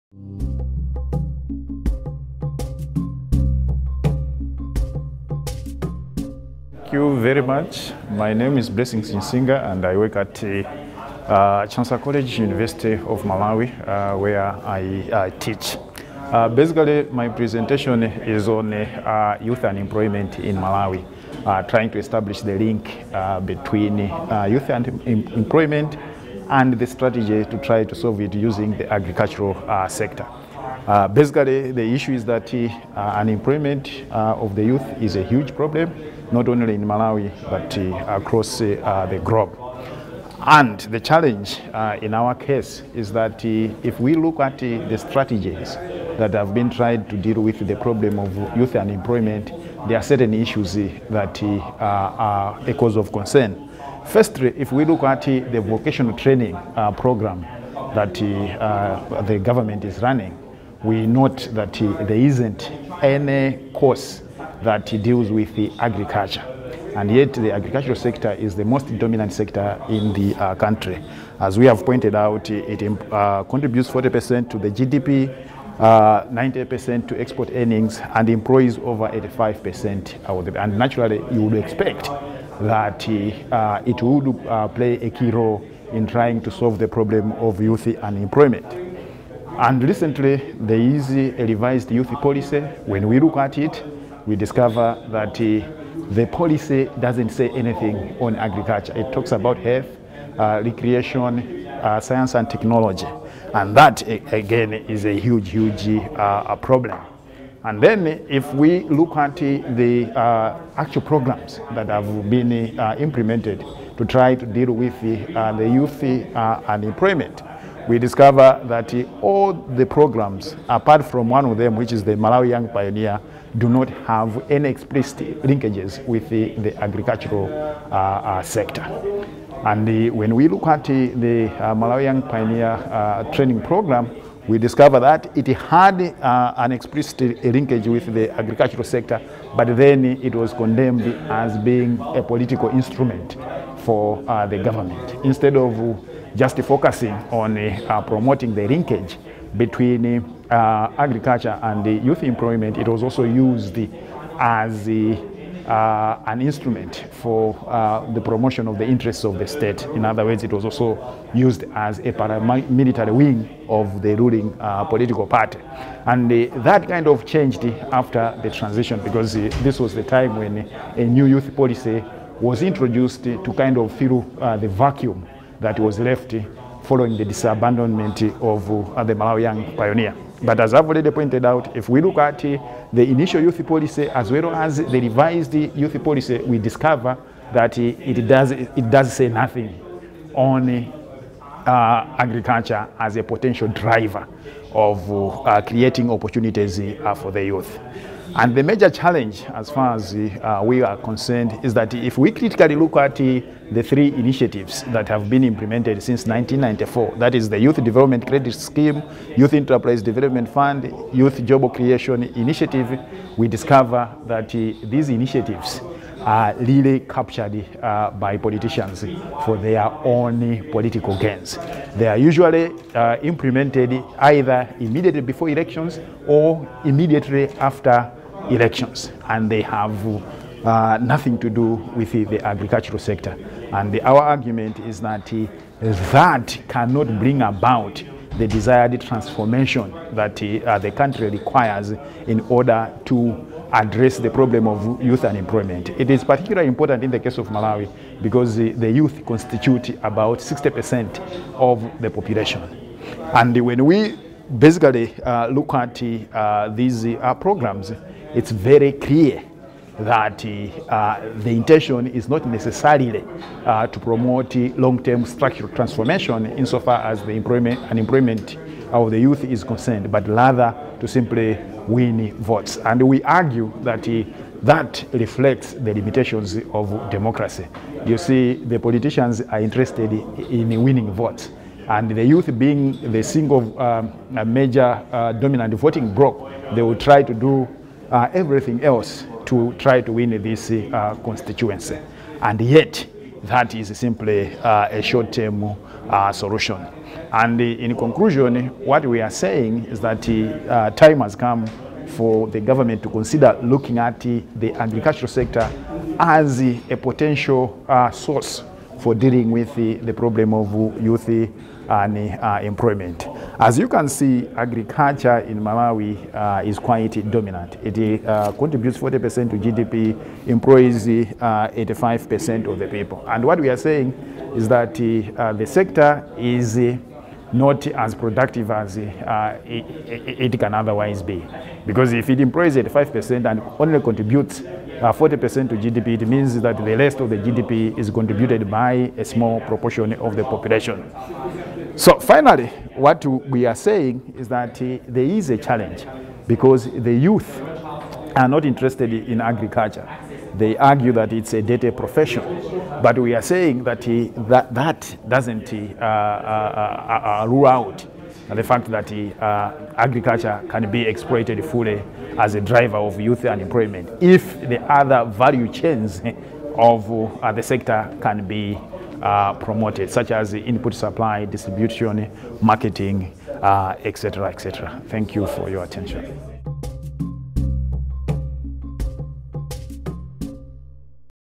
IGA Conference - Poster 1: Agriculture 1/2
Blessings Chinsinga: Searching for a Holy Grail? The nexus between agriculture and youth unemployment in Malawi

Inclusive Growth Conference: Measurement, Causes, and Consequences

The UNU-WIDER development conference in September 2013 focused on the performance, prospects, and policies for promoting more inclusive growth throughout Africa, including North Africa.